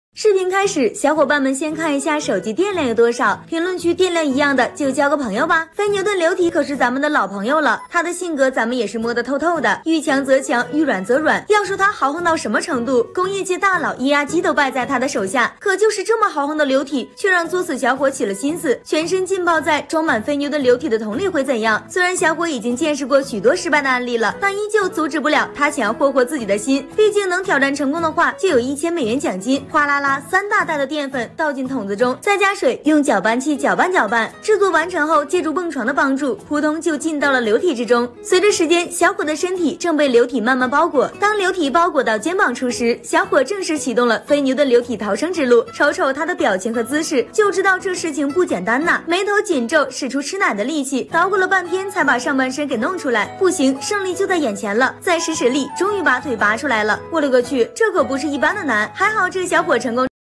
浸泡在非牛顿流体桶中会怎样？老外大胆挑战，结果肠子都悔青了！
浸泡在非牛顿流体桶中会怎样？老外大胆挑战，结果肠子都悔青了！,本视频由老彭带你看世界原创提供,4800次播放,好看视频是由百度团队打造的集内涵和颜值于一身的专业短视频聚合平台